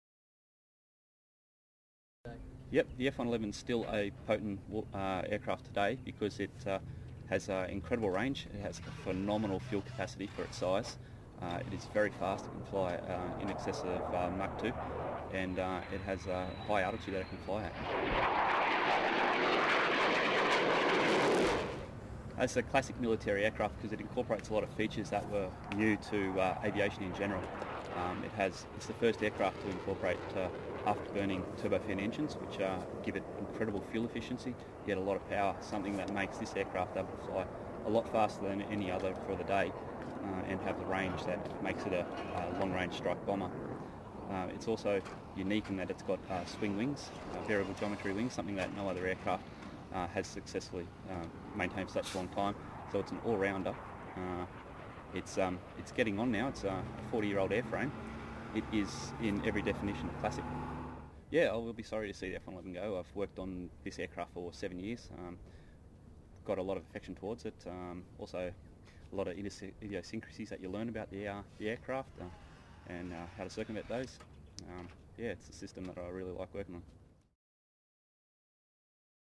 Pitch Black 2010 F111 3
Pitch Black 2010: Royal Australian Air Force Aircraft Technician Corporal Brad talks about the F-111 after working with them for nine years.